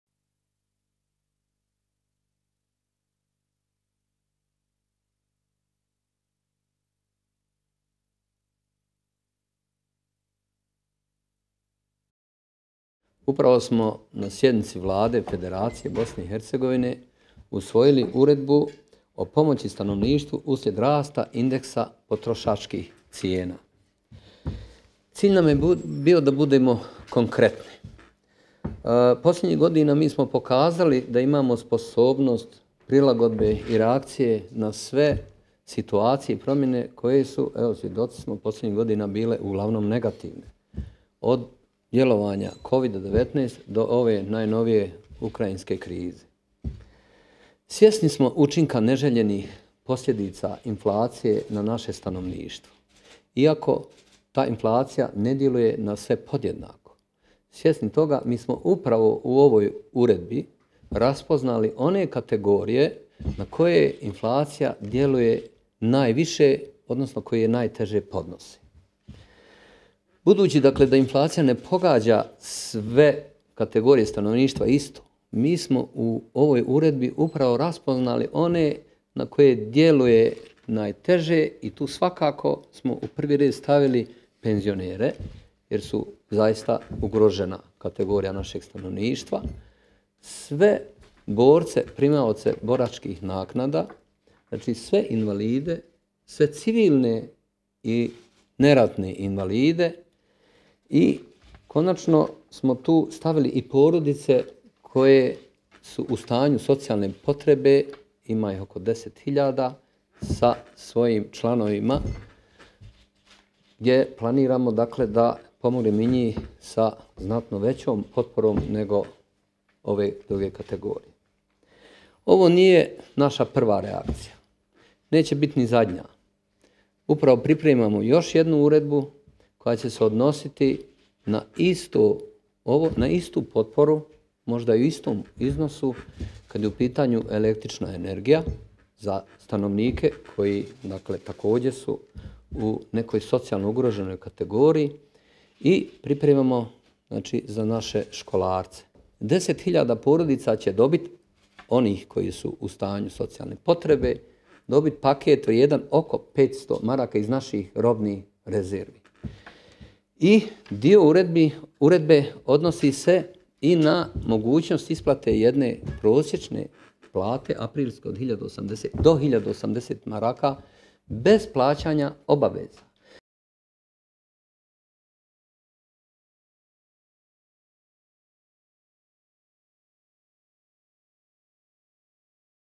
Novalić: Vladina Uredba sadrži niz mjera za podršku stanovništvu pogođenom inflacijom
SARAJEVO, 7. jula (FENA) - Vlada Federacije Bosne i Hercegovine usvojila je na današnjoj sjednici Uredbu o programu pomoći kategorijama stanovništva koje su posebno pogođene inflacijom i koja sadrži niz mjera usmjerenih na podršku stanovništva koji su pogođeni rastom indeksa potrošačkih cijena. (Video FENA/Harun Muminović)